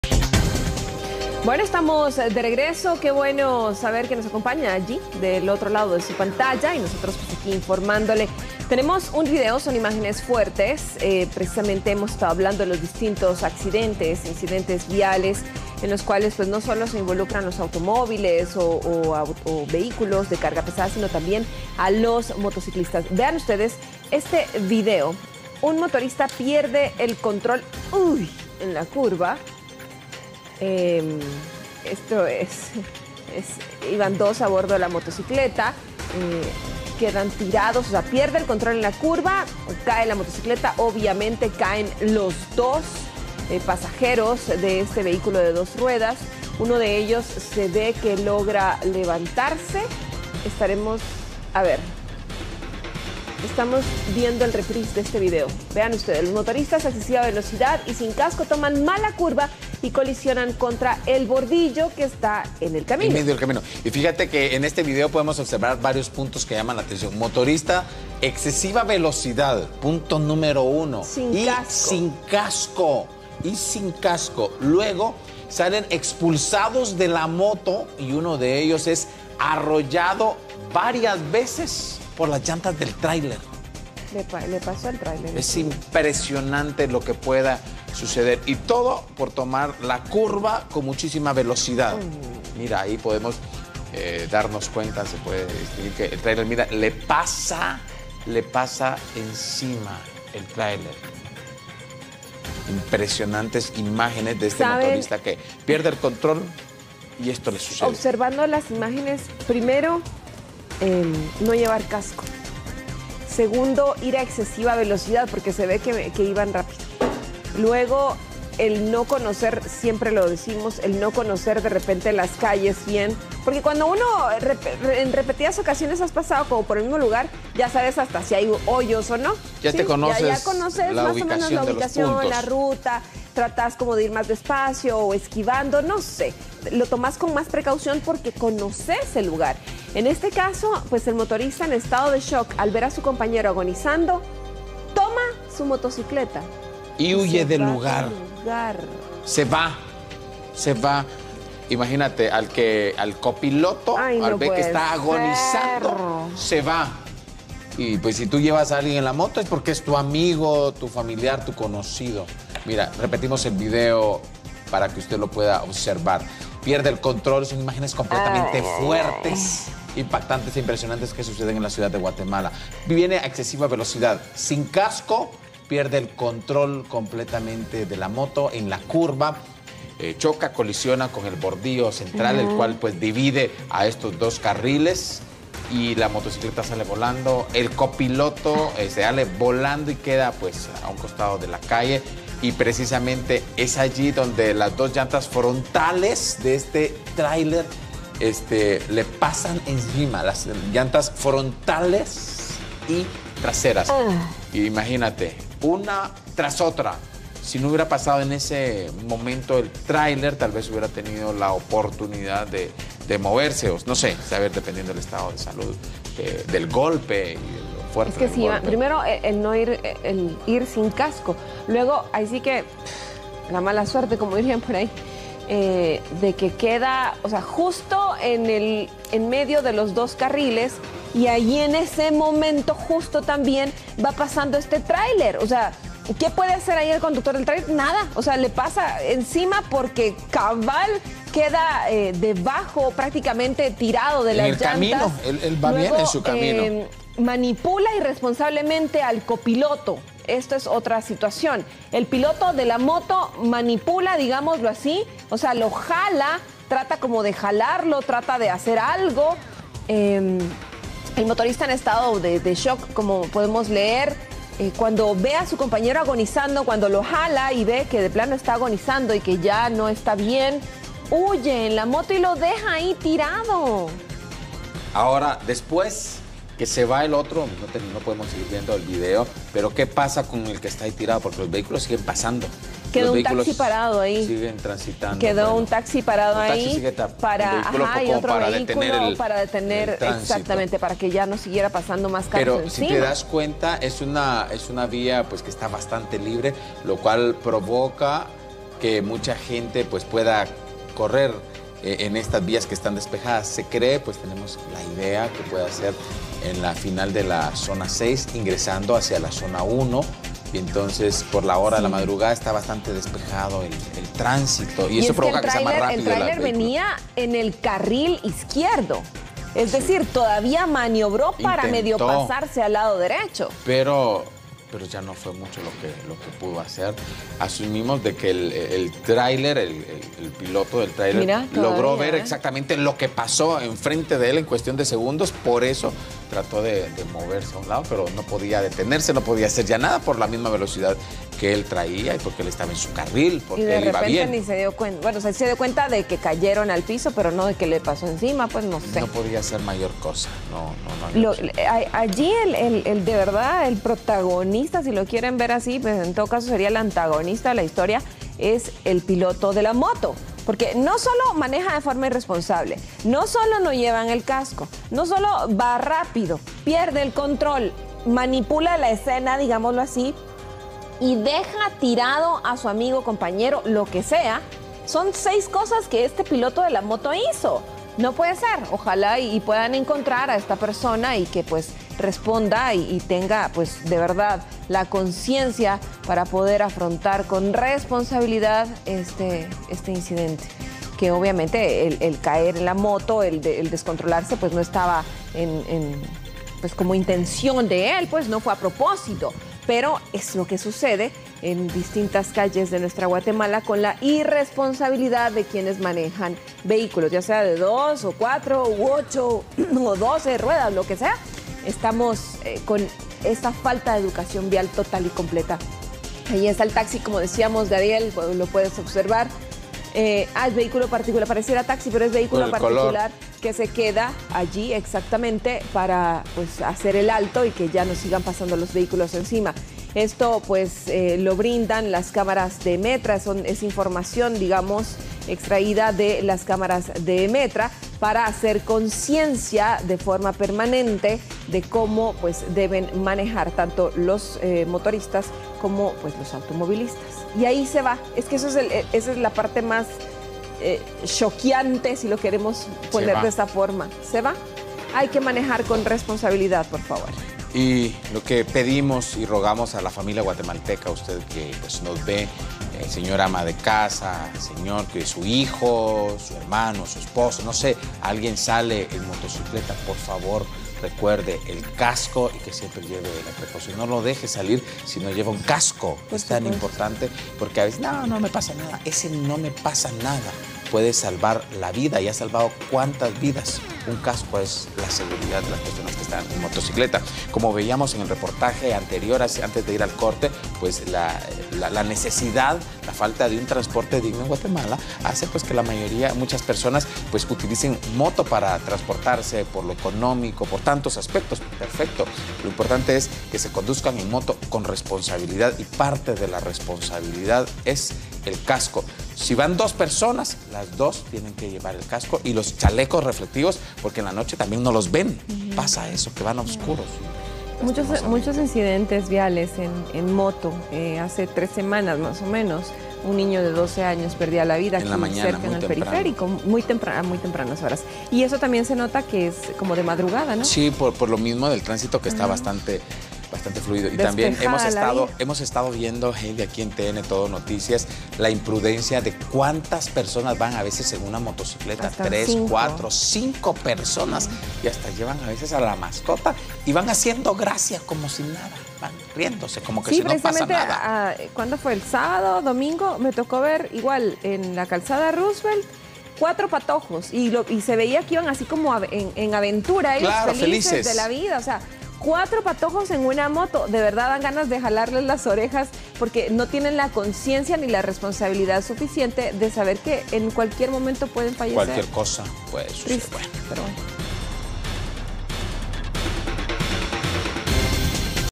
Motorista perdió el control en una curva
Se conducía a exceso de velocidad
El conductor colisionó contra el bordillo del camino, el copiloto salió expulsado del vehículo y fue arrollado varias veces por un tráiler; el piloto de la moto dejó a su compañero agonizando y se dio a la fuga. Imágenes fuertes.